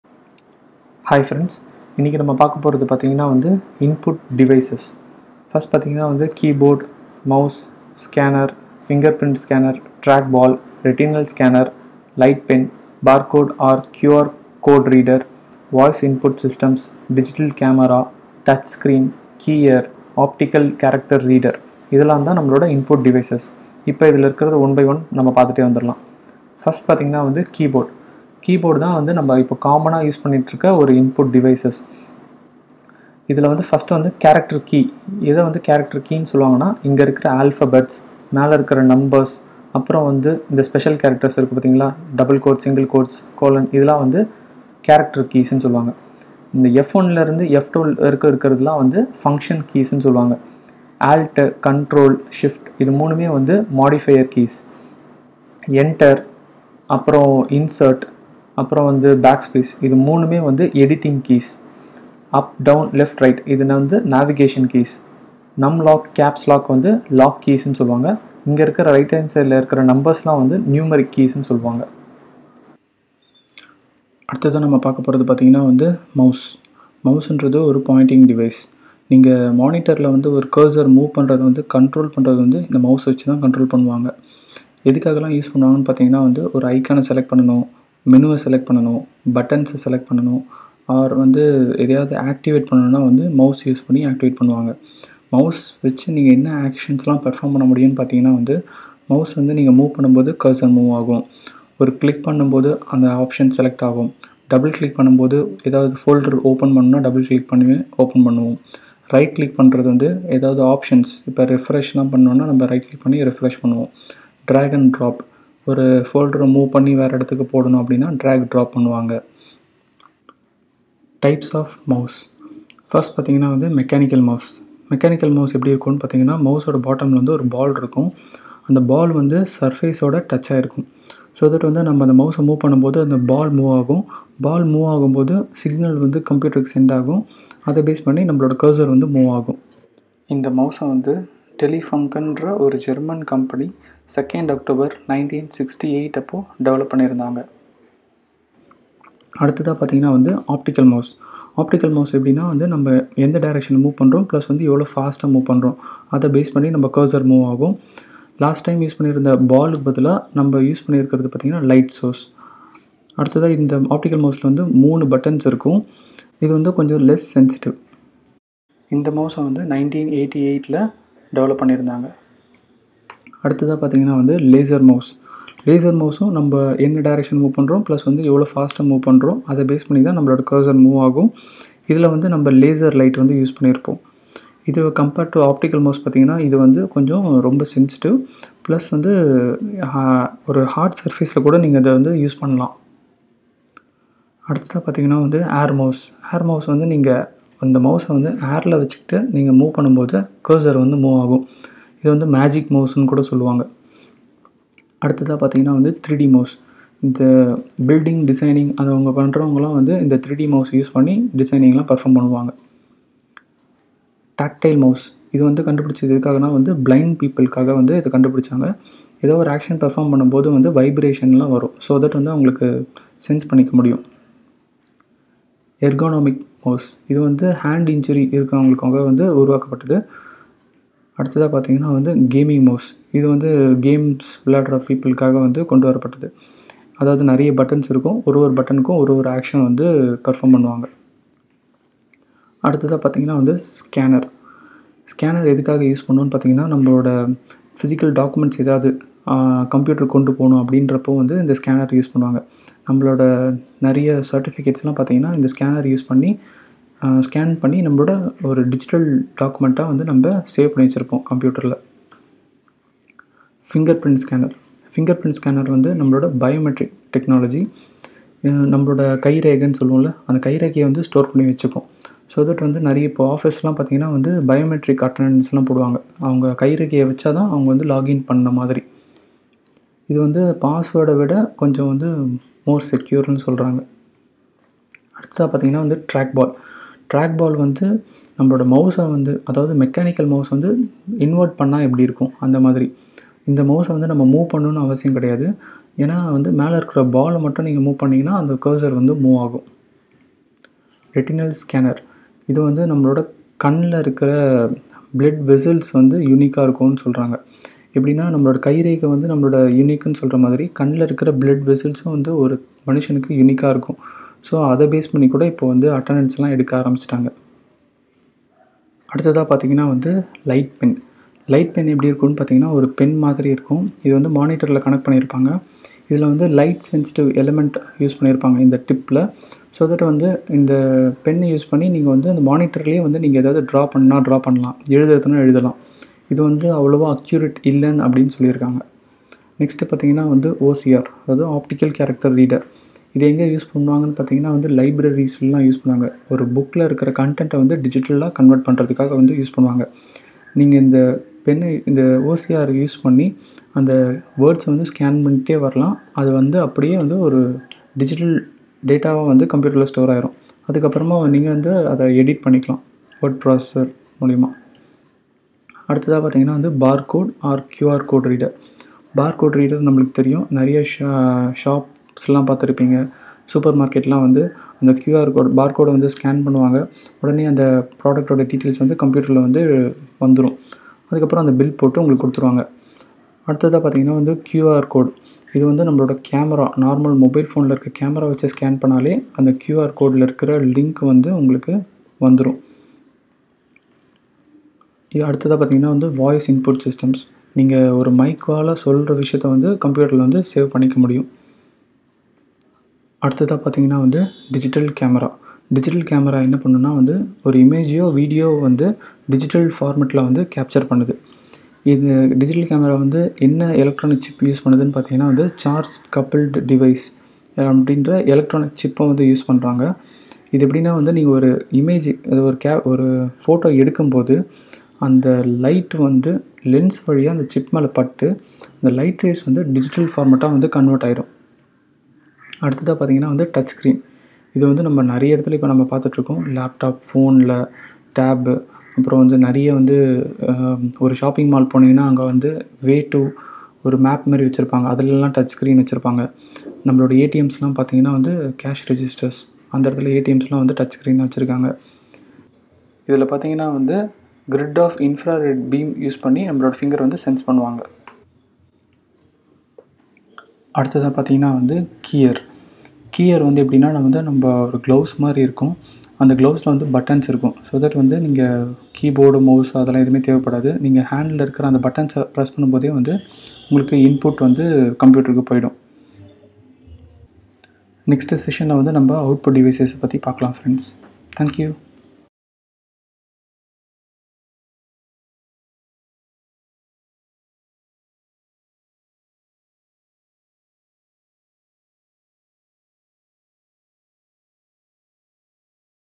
Input Devices of Computer CHAPTER 1 Part 5 | Tamil | Yogesh Education
Input Devices of Computer
* Keyboard
* Mouse
* Scanner
* Fingerprint scanner
* Track Ball
* Retinal scanner
* Light pen
* Barcode / QR Code reader
* Voice Input Systems
* Digital Camera
* Touch Screen
* Keyer
* Optical Character Reader